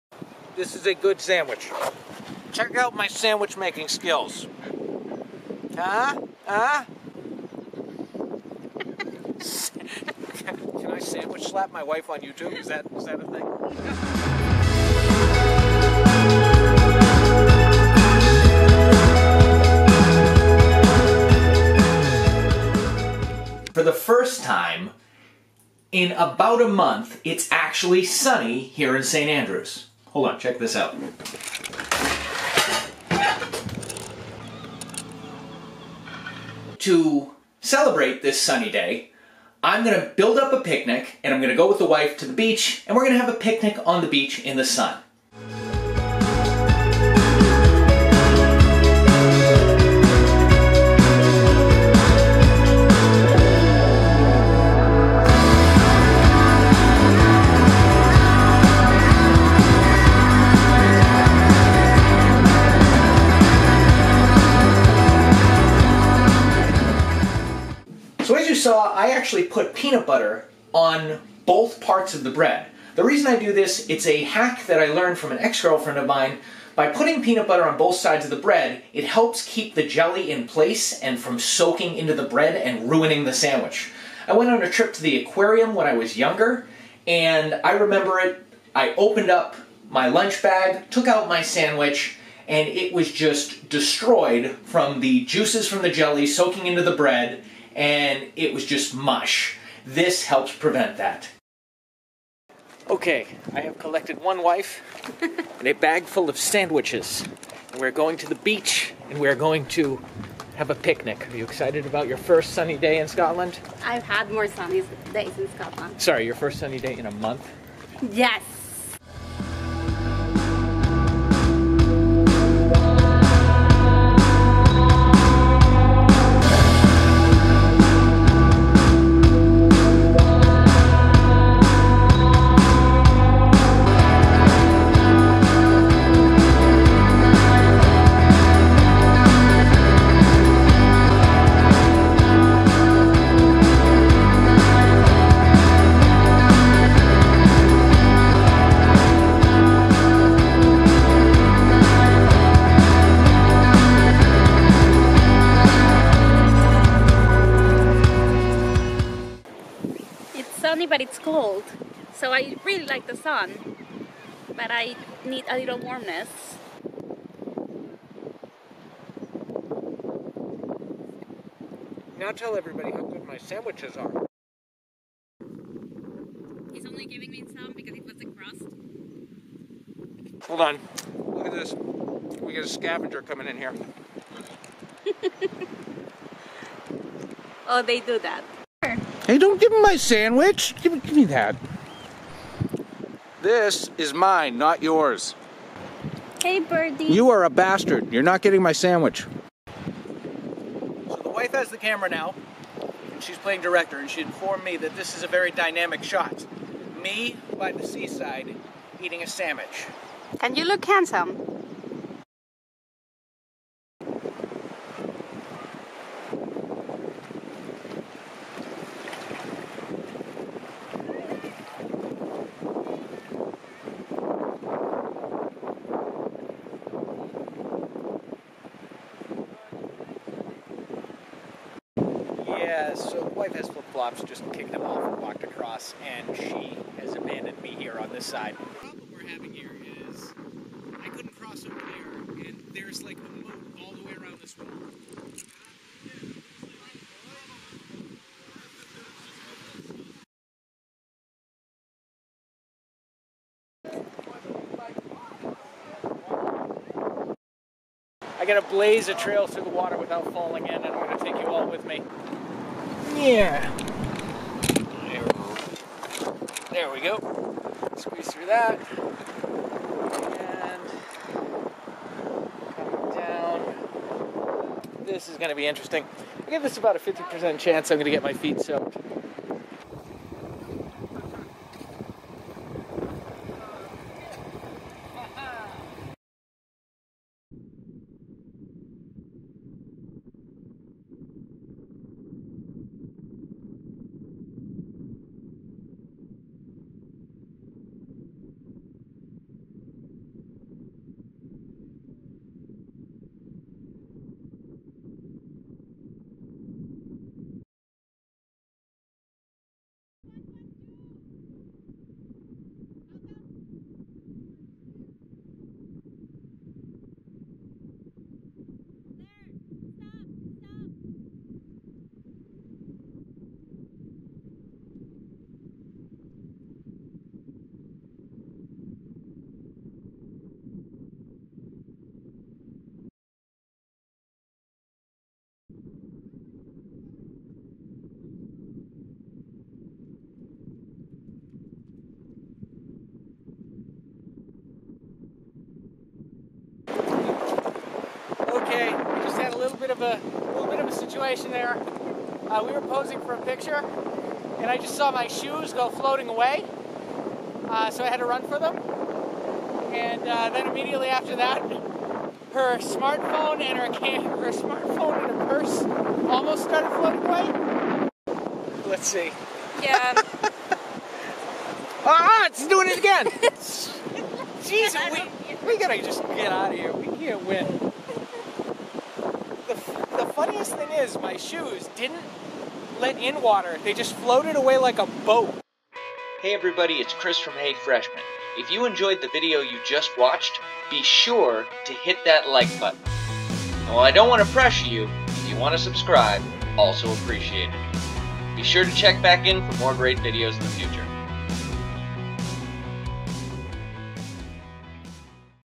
Flooded Camera Gear | Vlog 163
First sunny day in St. Andrews this month! I pack up my camera gear and a picnic lunch and head to the beach the wife.  We were so excited to enjoy the sun we waded into the water paying no attention to the tide line... until I saw my shoes floating away and my camera gear being flooded!